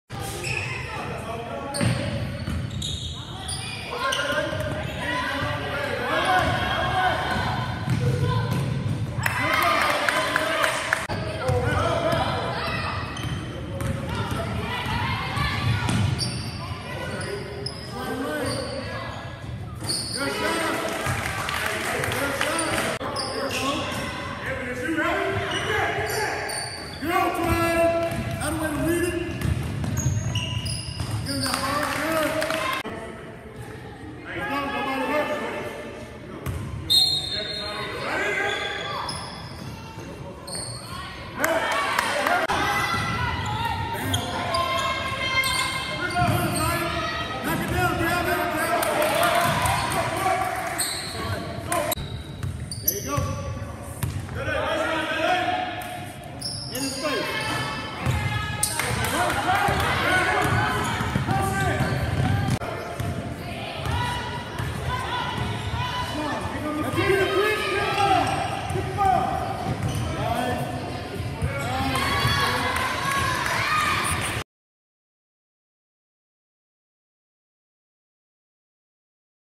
Bayou City Hoops (TX) picks up the 59-42 win against HTB (TX) - Kingwood Classic
Kingwood Classic.